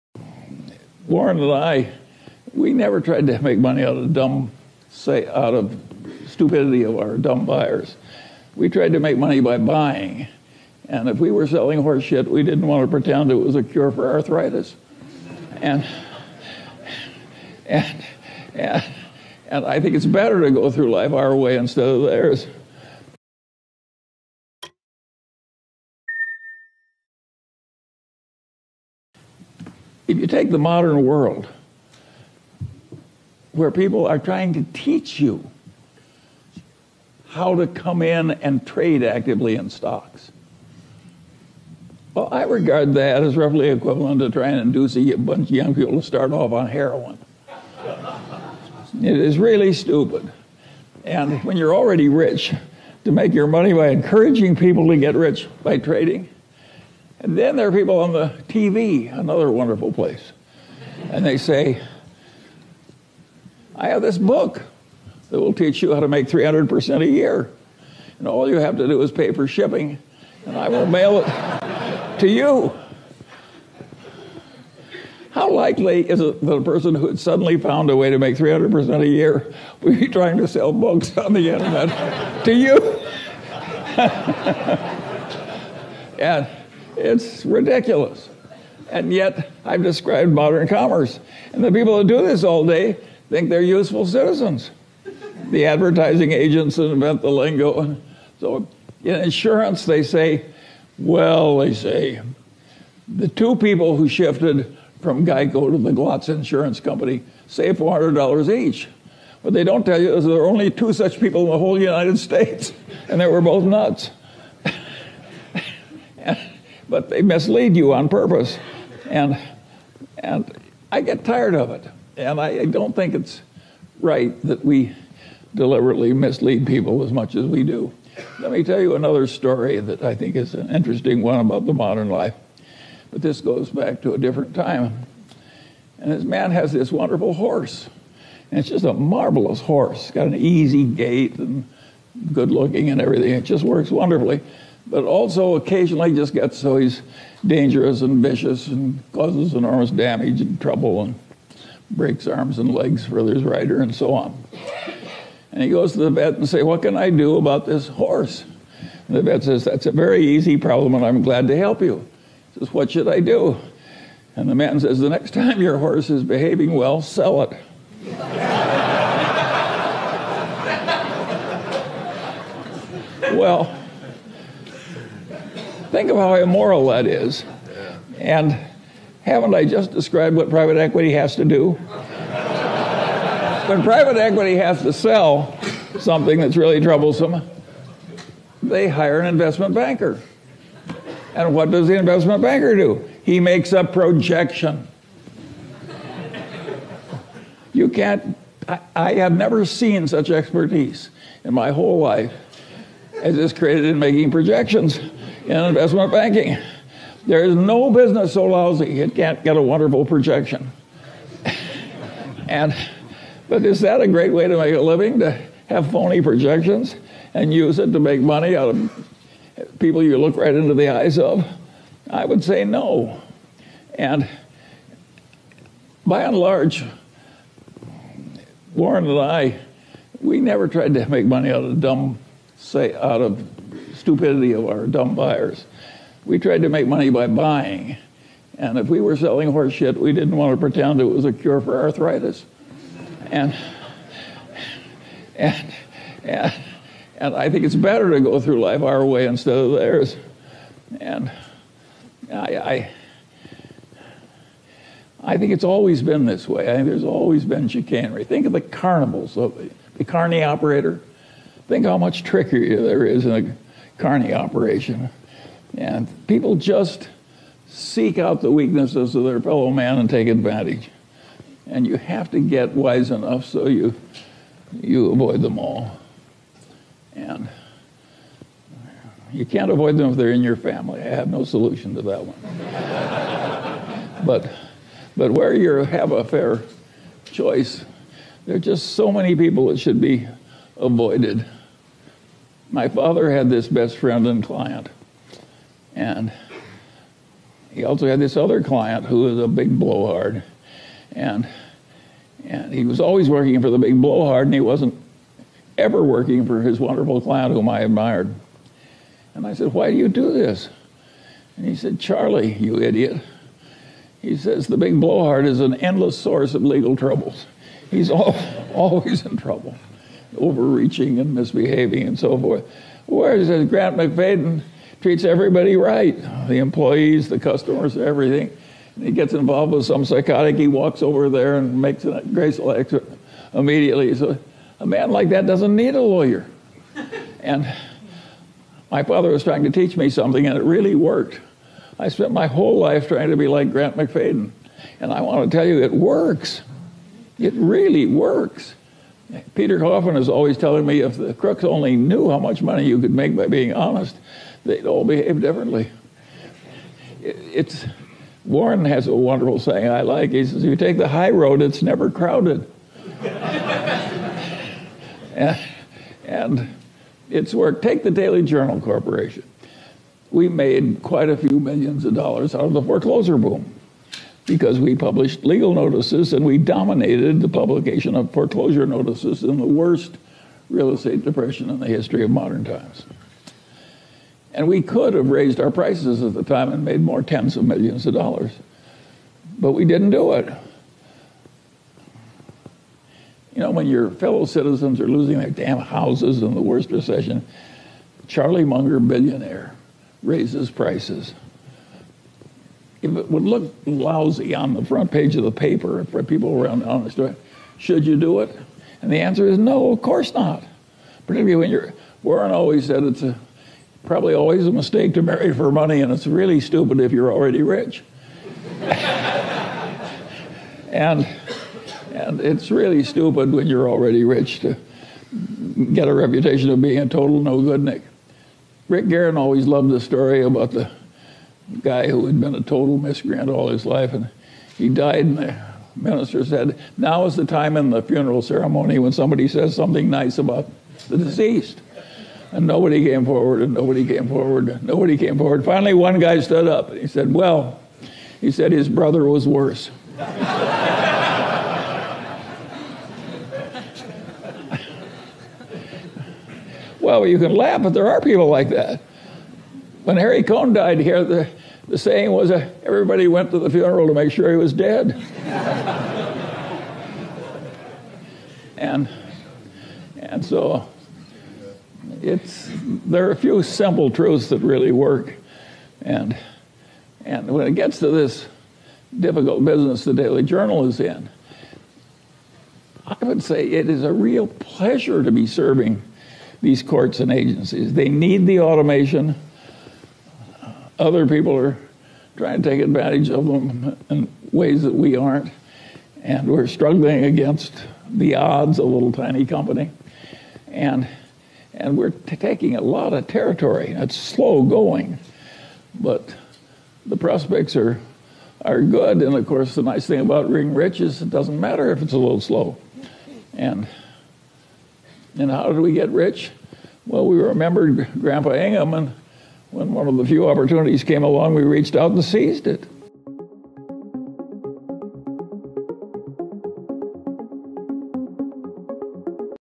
Charlie Munger's Advice On How To Become a Billionaire
Charlie Munger is an American investor, businessman, former real estate attorney, architectural designer, and philanthropist.

Welcome to Investor Lounge where you can find investing advice from some of the greatest (and wealthiest) investors in the world.